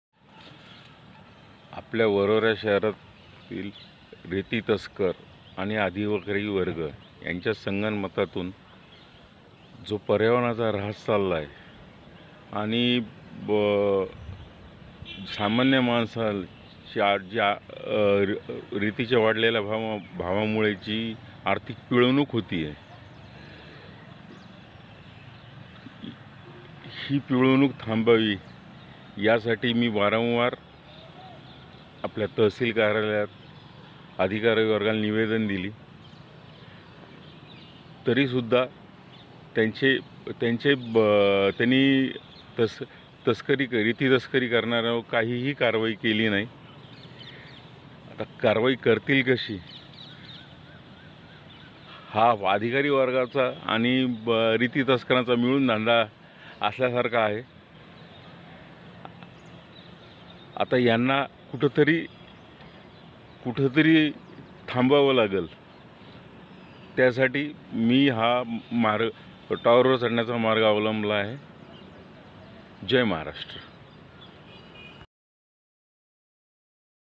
रेती तस्करी विरोधात मनसे तालुका अध्यक्षाची विरुगीरी
वरोरा तालुक्यात खुलेआम रेती चोरी विरोधात मनसेचे टॉवरवर चढून अभिनव आंदोलन